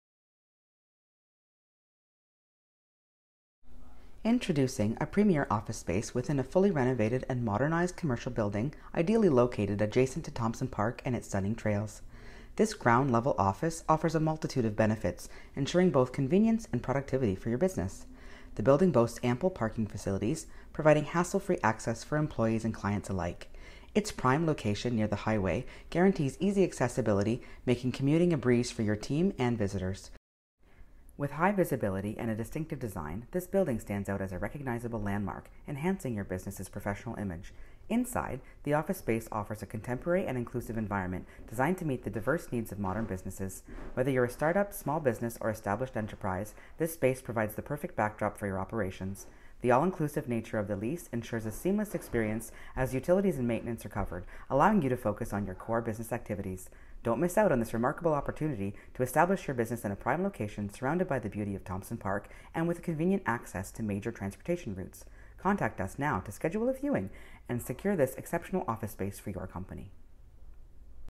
Commercial Lease: 1305 Fisher St. - North Bay Units #107 & #117
Introducing a premier office space within a fully renovated and modernized commercial building, ideally located adjacent to Thompson Park and its stunning trails. This ground-level office offers a multitude of benefits, ensuring both convenience and productivity for your business. The building boasts ample parking facilities, providing hassle-free access for employees and clients alike. Its prime location near the highway guarantees easy accessibility, making commuting a breeze for your team and visitors. With high visibility and a distinctive design, this building stands out as a recognizable landmark, enhancing your business's professional image. Inside, the office space offers a contemporary and inclusive environment, designed to meet the diverse needs of modern businesses. Whether you're a startup, small business, or established enterprise, this space provides the perfect backdrop for your operations. The all-inclusive nature of the lease ensures a seamless experience, as utilities and maintenance are covered, allowing you to focus on your core business activities. Don't miss out on this remarkable opportunity to establish your business in a prime location, surrounded by the beauty of Thompson Park and with convenient access to major transportation routes.

Mike Burman
Broker of Record